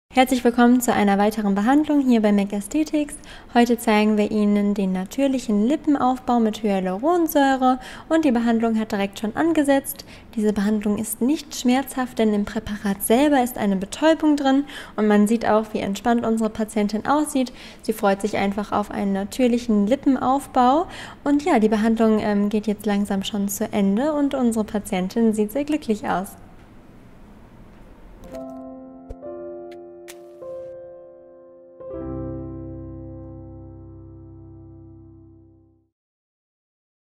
Mc Aesthetics präsentiert den Lippenaufbau
Ashe L. aus Mannheim schreibt:,, Lippen aufspritzen zu lassen ist nicht für das man sich schämen sollte, ganz im Gegenteil, ich habe die Behandlung gemacht und ich finde sie super. Jede Frau oder jeder Mann der sich die Lippen etwas voller wünscht, sollte die Behandlung durchführen lassen. Ich bin wirklich gespannt auf meinen nächsten Besuch und bis dahin alles gute und viel Gesundheit.